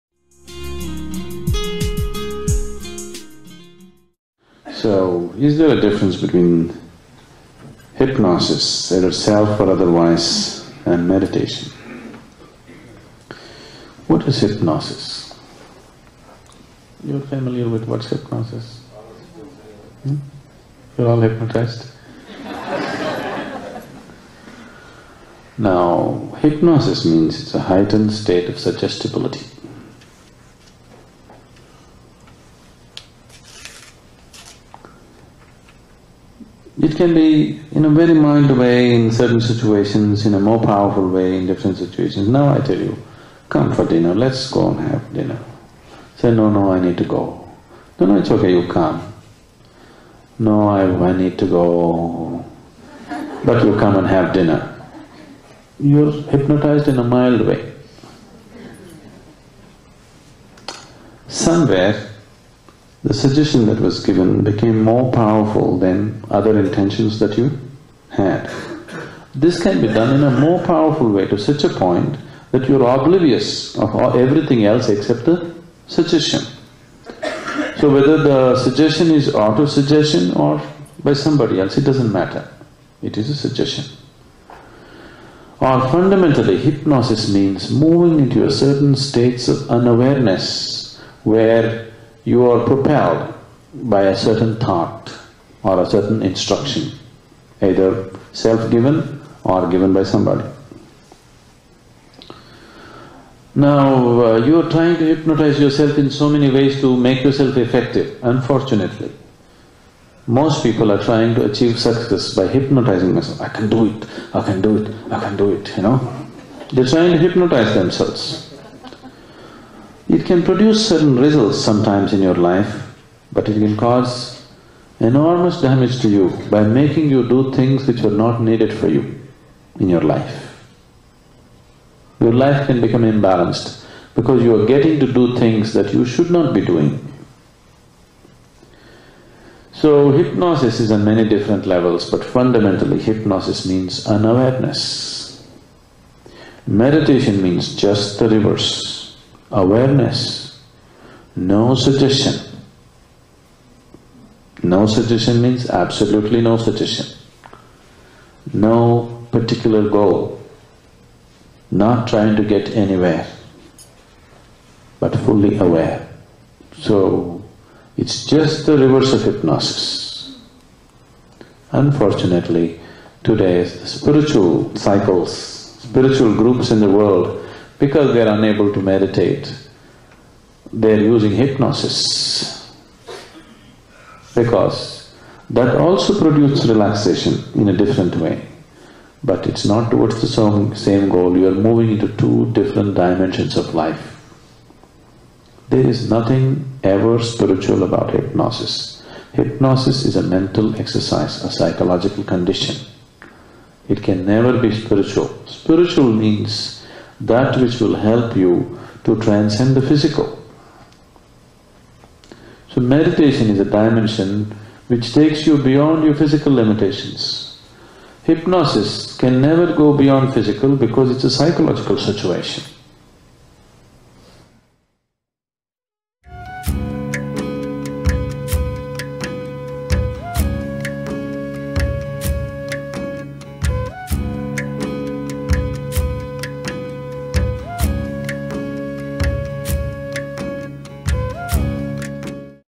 Che cos'è l'ipnosi e in che modo si differenzia dalla meditazione? | Sadhguru Italiano
In questo video Sadhguru spiega la differenza fondamentale tra l'ipnosi e la meditazione.